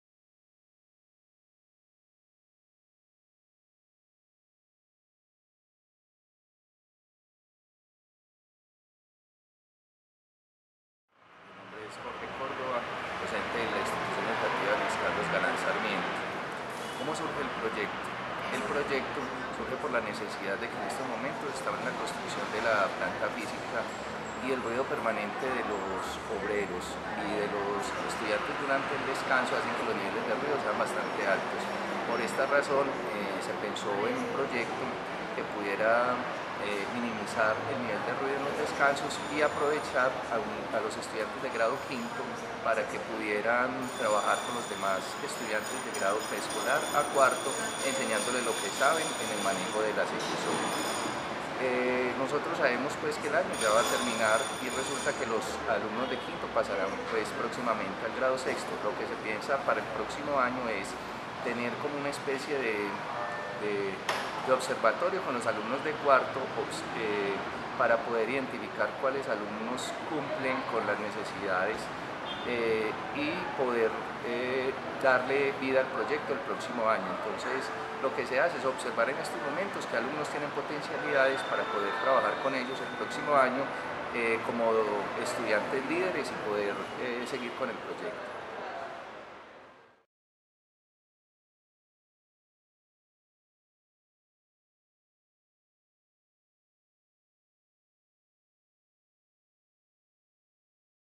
Jorge Córdoba: Sonidos Pedagógicos
Luego de las presentaciones en el Encuentro TESO, los docentes respondieron algunas preguntas acerca de sus ponencias.
Jorge Córdoba nos cuenta cómo surge el Sonidos Pedagógicos y la estrategia para garantizar su continuidad en años venideros.